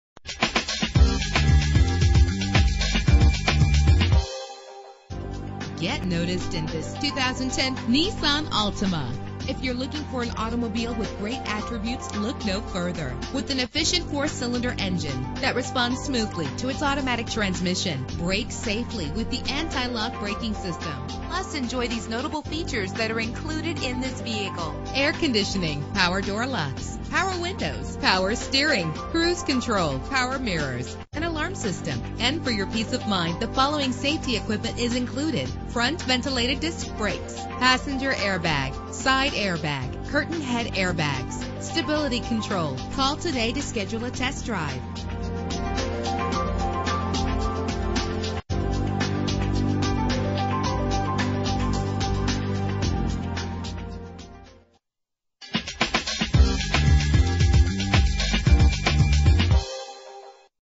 2010 Nissan Altima - Elizabethtown KY
Year: 2010
Make: Nissan
Model: Altima
Trim: 2.5
Engine: 2.5 liter inline 4 cylinder 16 valve
Transmission: Automatic CVT
Color: Radiant Silver
Mileage: 44522
Address:
1085 North Dixie Highway